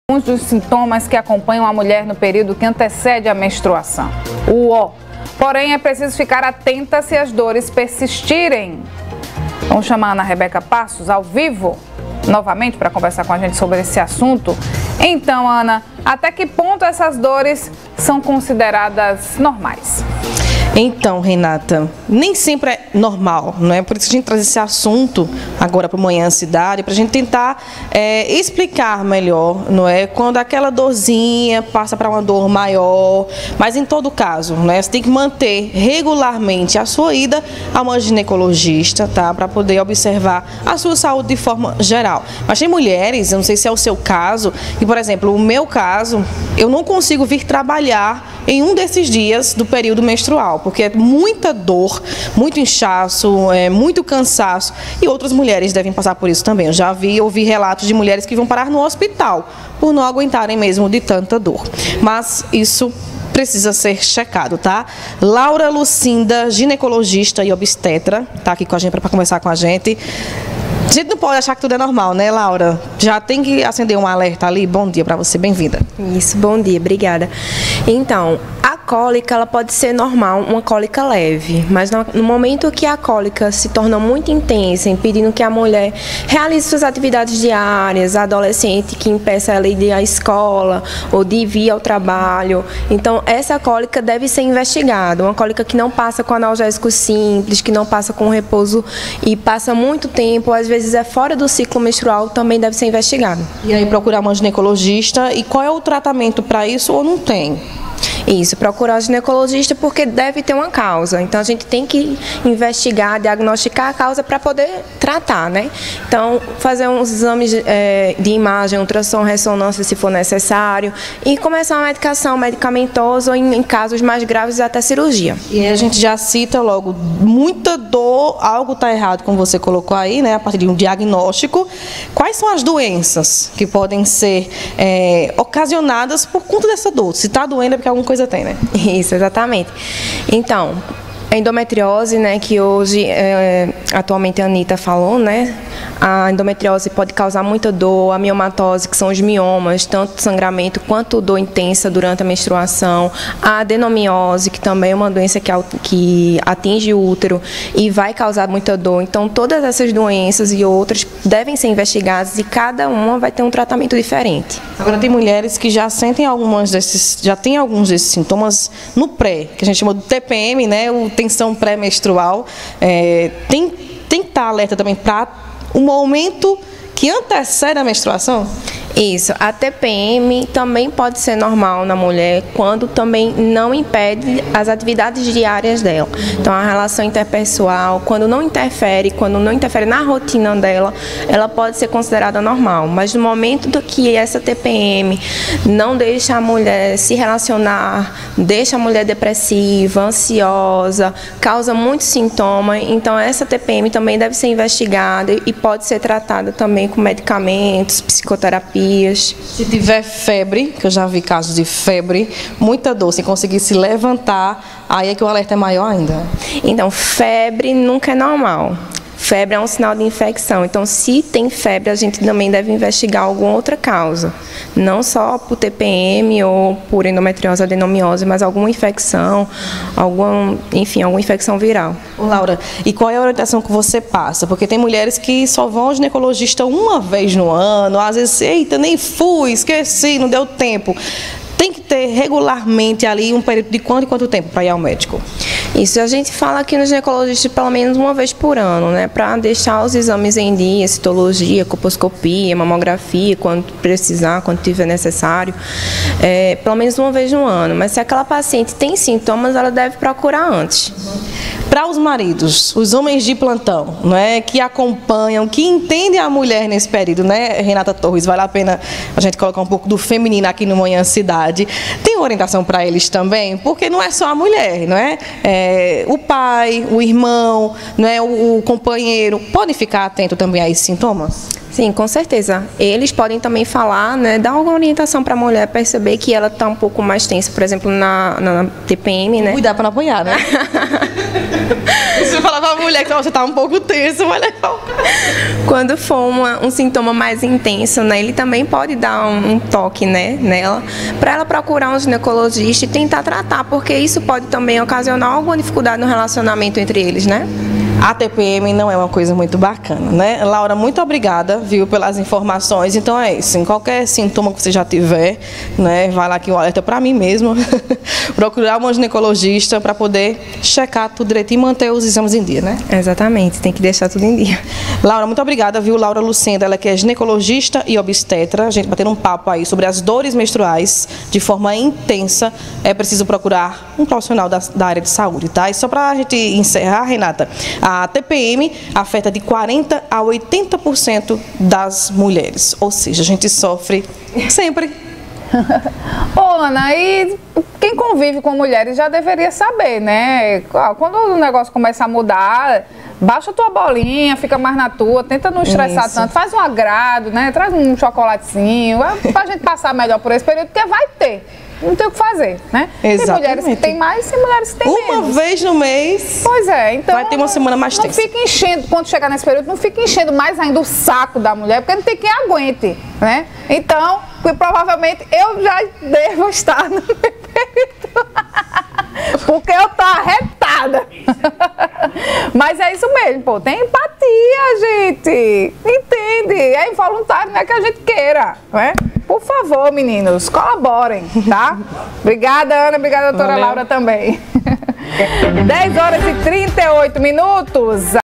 Manhã Cidade - 06.10.22 - Dores durante a menstruação
Sobre esse assunto, a repórter Ana Rebeca Passos conversa com a ginecologista Laura Lucinda.